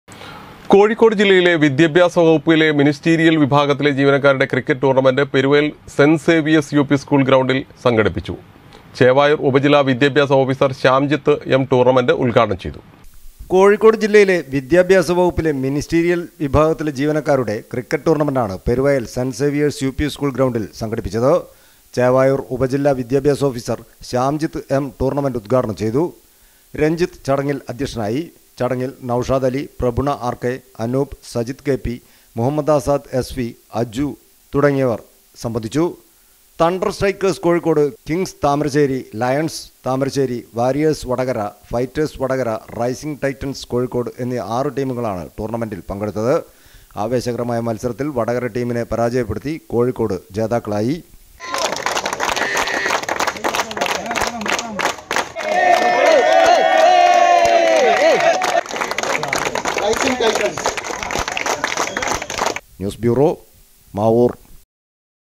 കോഴിക്കോട് ജില്ലയിലെ വിദ്യാഭ്യാസ വകുപ്പിലെ മിനിസ്റ്റീരിയൽ വിഭാഗത്തിലെ ജീവനക്കാരുടെ ക്രിക്കറ്റ്
കോഴിക്കോട് ജില്ലയിലെ വിദ്യാഭ്യാസ വകുപ്പിലെ മിനിസ്റ്റീരിയൽ വിഭാഗത്തിലെ ജീവനക്കാരുടെ ക്രിക്കറ്റ് ടൂർണമെണ്ട് പെരുവയൽ സെന്റ് സേവിയേഴ്സ് യുപി സ്കൂൾ ഗ്രൗണ്ടിൽ സംഘടിപ്പിച്ചു.